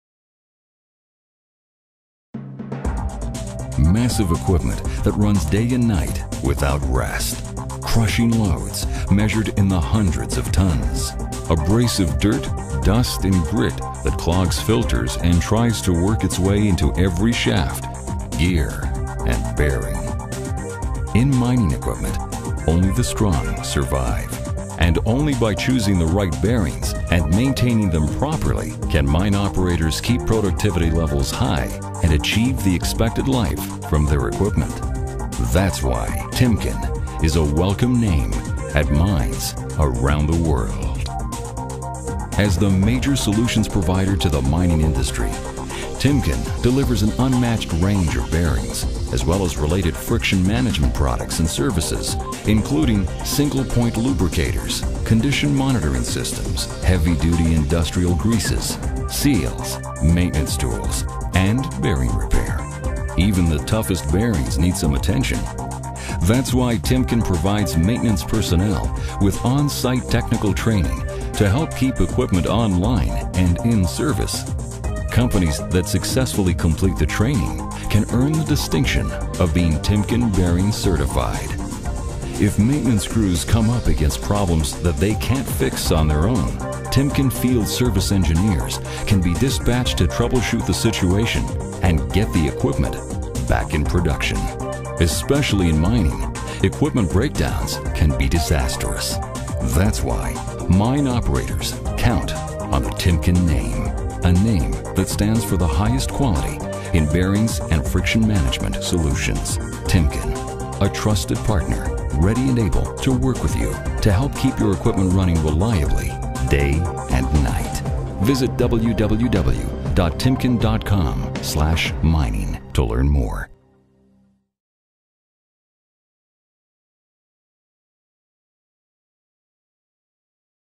Timken Solutions for Above & Underground Mining l SLS Partner Timken
Surface and underground mining equipment face challenges like dirt, dust, and crushing loads every day. Mining machinery runs around the clock. Draglines, haul trucks, and underground equipment like long wall shearers and continuous mining machines demand reliability and efficiency.

Timken is a recognized world leader in friction management, especially in harsh operating environments. Timken application and engineering expertise, advanced bearing design and manufacturing, testing solutions, maintenance training, lubrication solutions and more meet the challenges mining operations face. Timken is a trusted partner to mining equipment manufactures and mine operators.